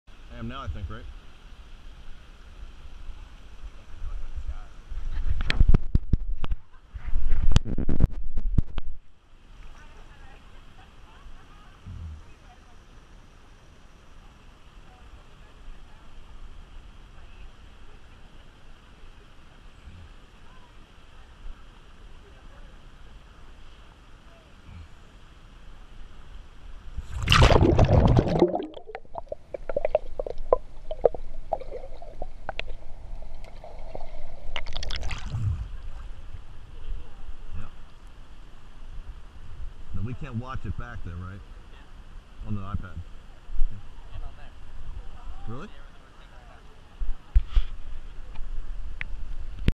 Gopro pool testing amazing quality
Vacationing with a new gopro, what a difference it will make on our vacations